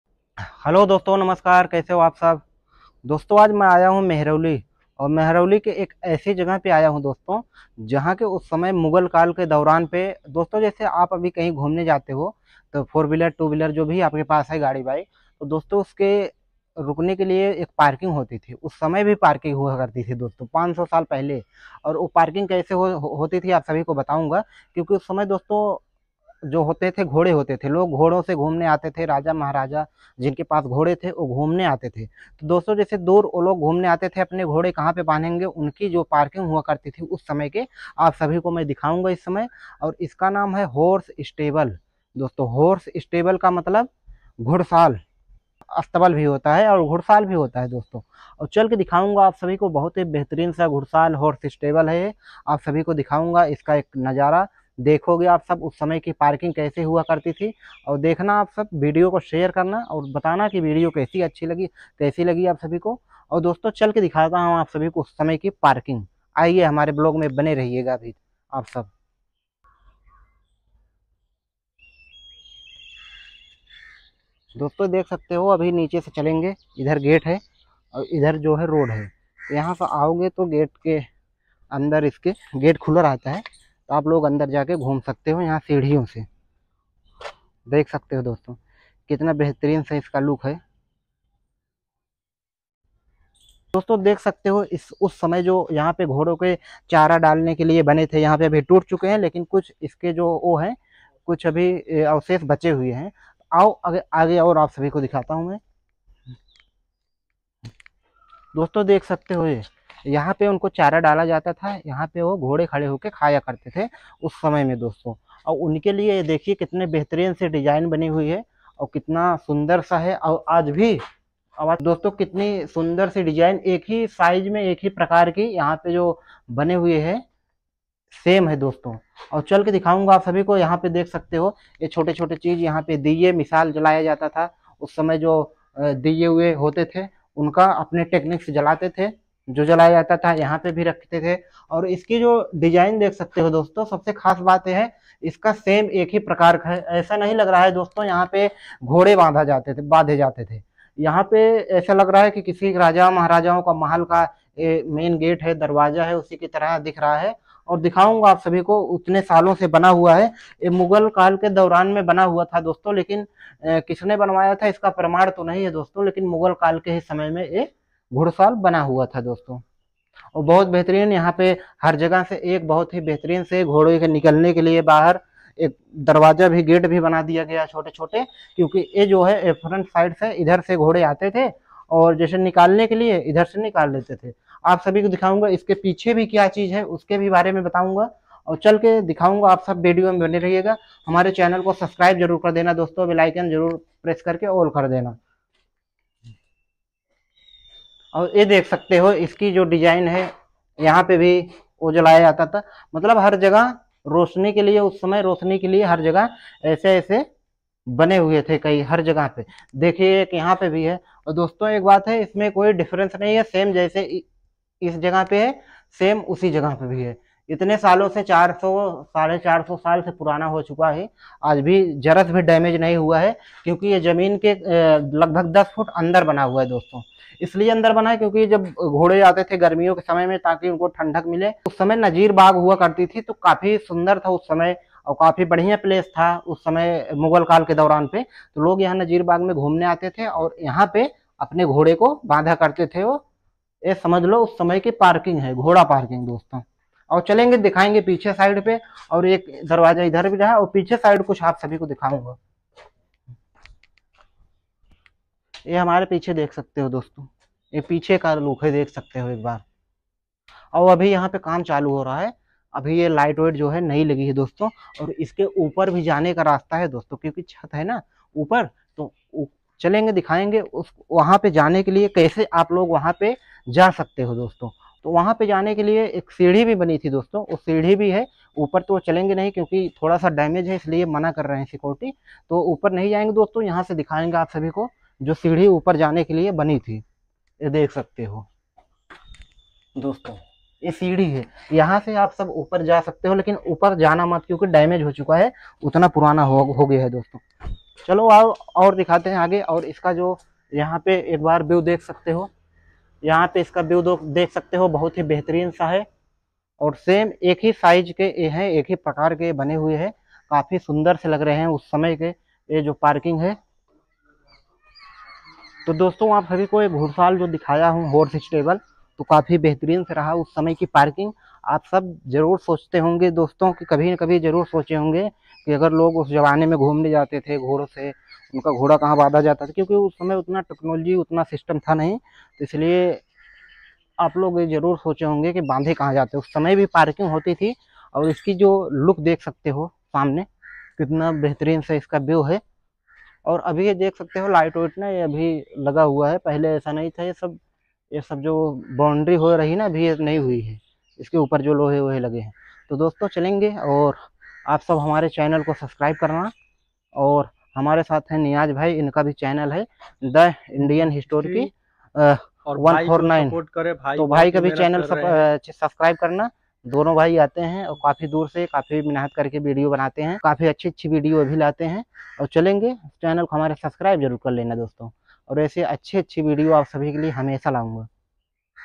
Mughal Kal ka Ghudhsaar | मुगल काल के समय का राजा महाराजाओ का घोड़ा पार्किंग घुड़सार / horse steble
Dosto aap sabhi ko bta du ki ghudhsar ka matlab stabale hota hai aur hours steble bhi hota hai ye ghursar dilli ke mehrauli me mughal kal ke samay ka hai dosto pahle ke samay jab raja mahraja ghumne aya karte the tab unke ghode ko badhne ke liye banaya gya tha us samay ka parking hai aaj bhi bahut achha hai ye bahut sundar sa hai kisne banwaya tha ye pta nahi lekin eska praman hai ki mughal kal ke samay me bana tha

Address: kutubminar metro 🚊 station